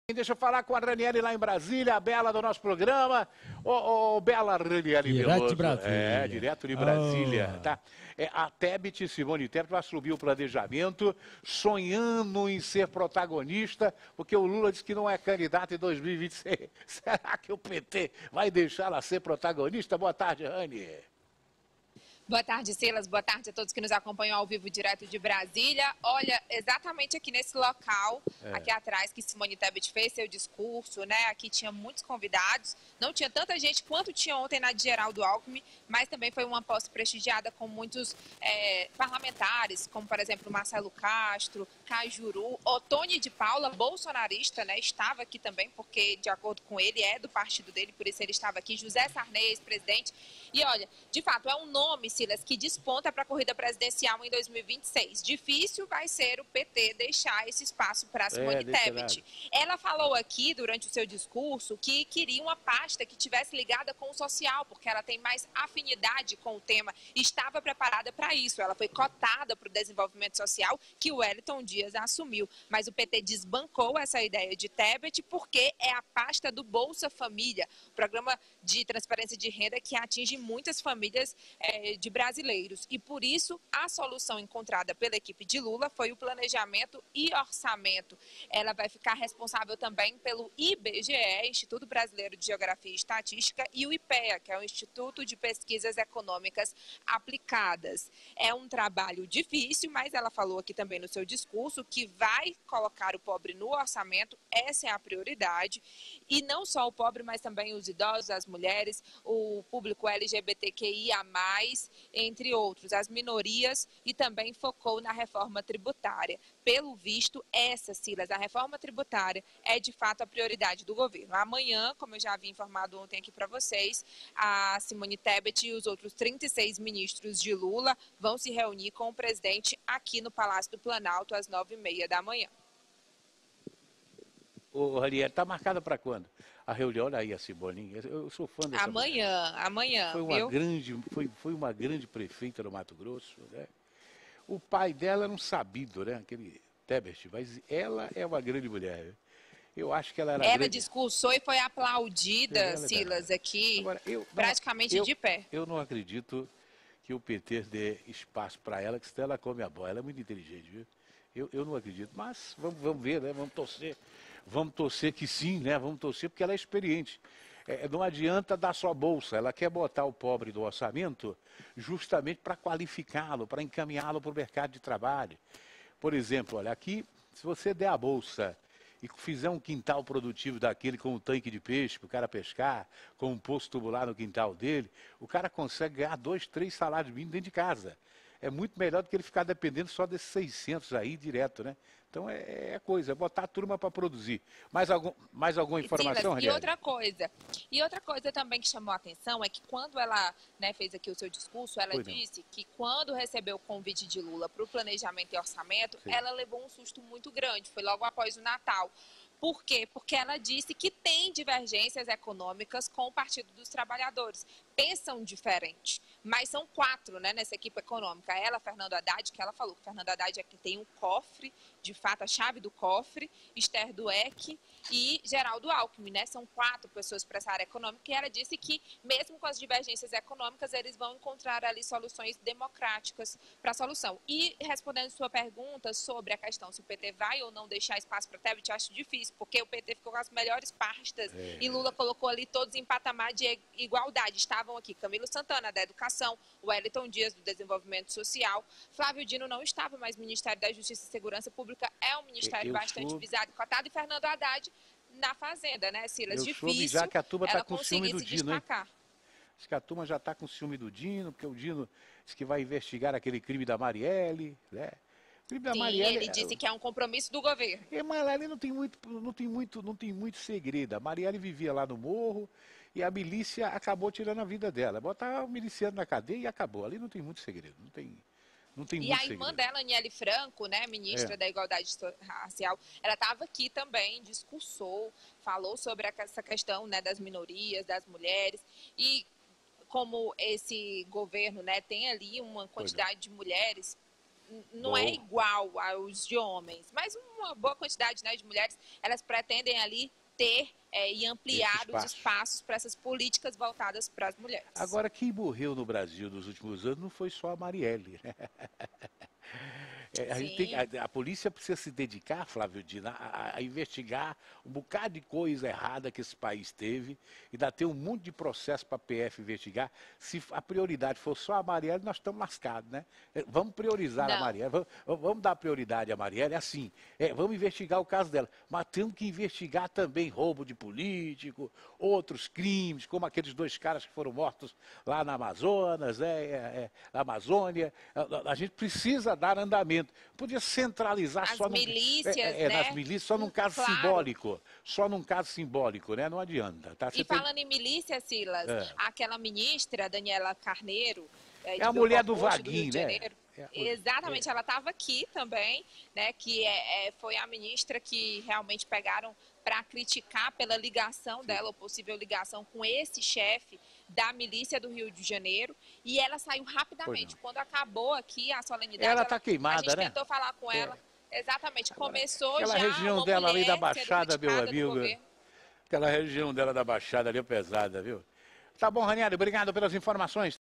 Tebet assume Planejamento de olho em protagonismo para 2026
A senadora Simone Tebet (MDB-MS) assumiu nesta quinta-feira (5) o cargo de ministra do Planejamento. O ministério foi recriado pelo presidente Luiz Inácio Lula da Silva (PT). Na gestão anterior, de Jair Bolsonaro (PL), a pasta fazia parte do Ministério da Economia, que foi desmembrado.